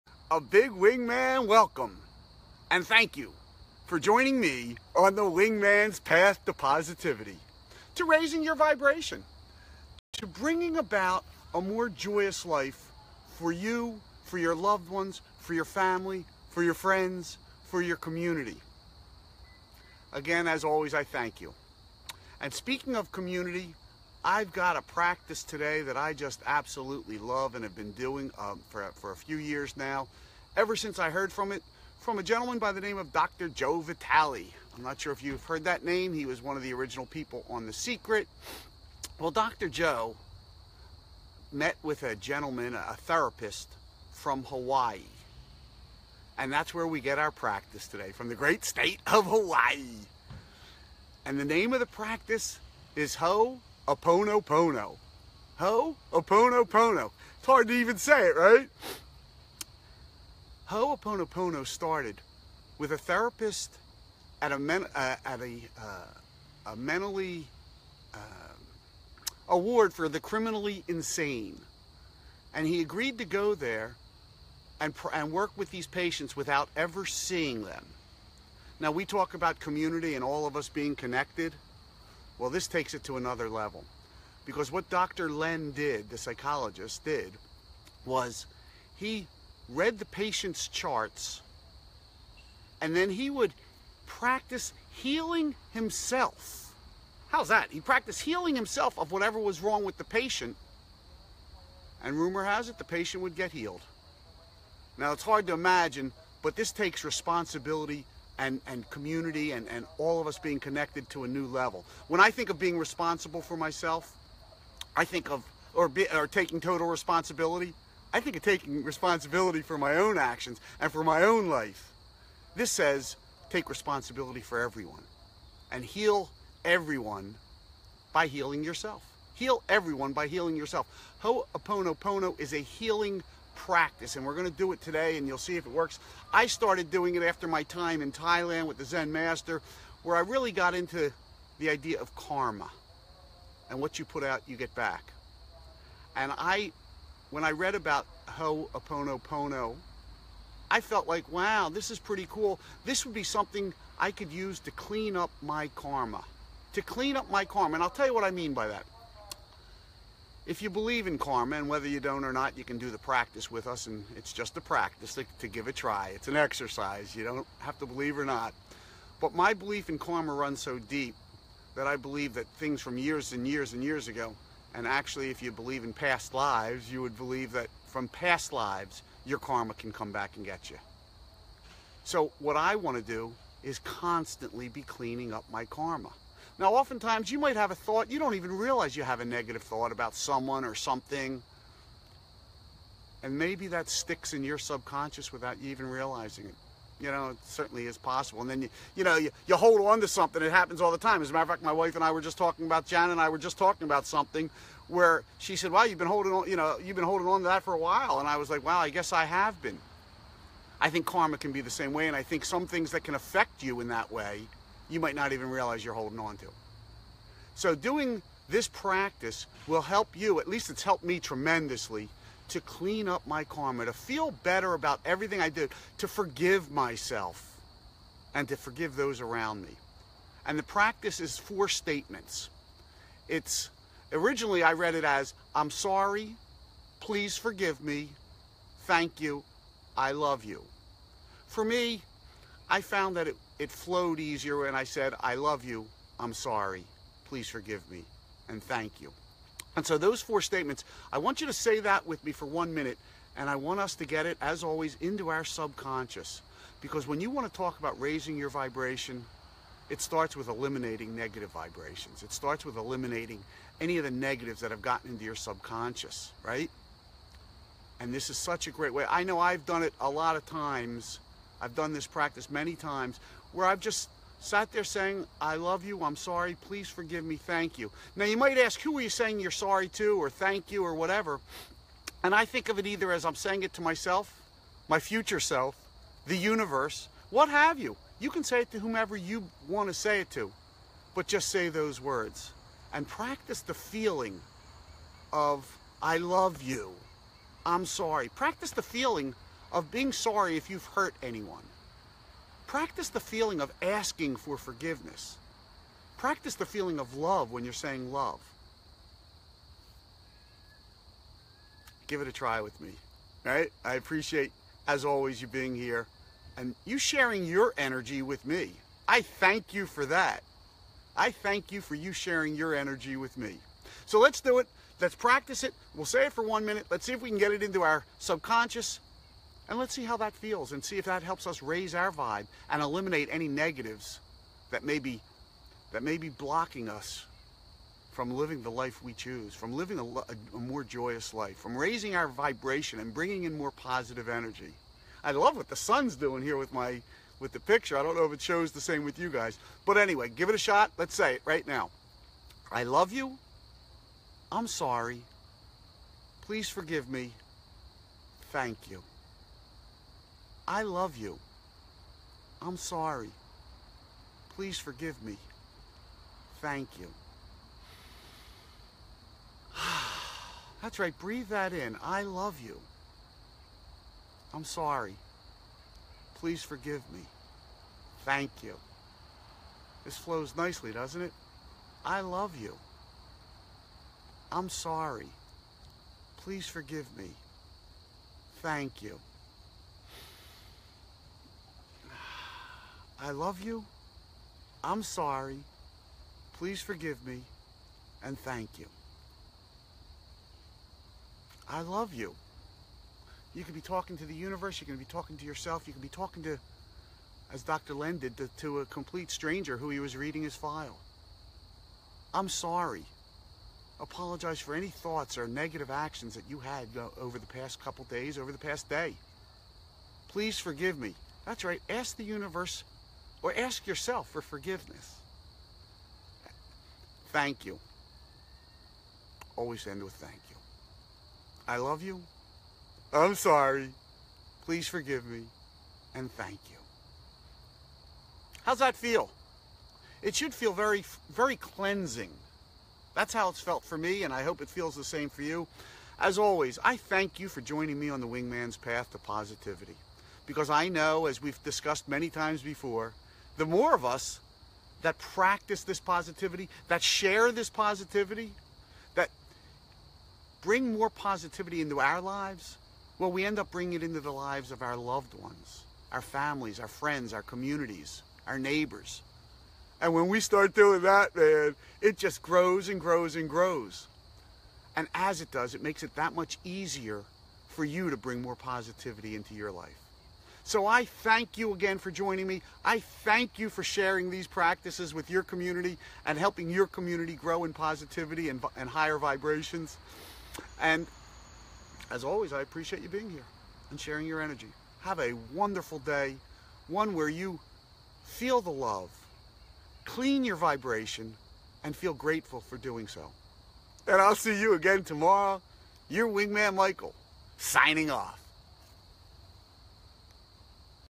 Ho'oponopono
Michael Gross, of beyourownwingman.com, shares a practice for cleaning up your karma.